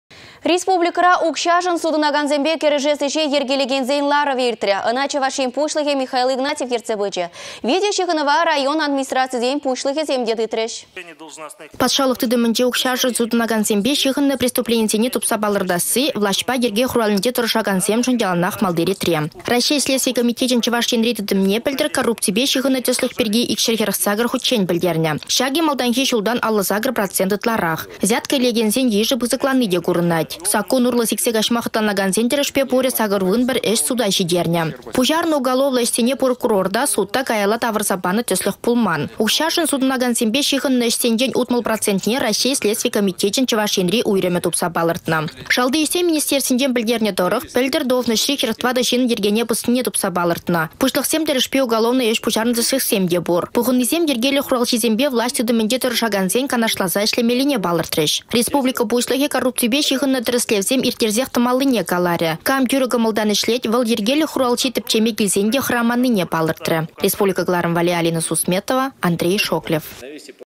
Коррупципе кĕрешес ĕçе йĕркелекен комиссин ларăвĕ
Республикăра укçашăн сутăнакансемпе кĕрешес ĕçе йĕркелекенсен ларăвĕ иртрĕ. Ăна Чăваш Ен Пуçлăхĕ Михаил Игнатьев ертсе пычĕ. Видеоçыхăнăва район администрацийĕсен пуçлăхĕсем те тытрĕç.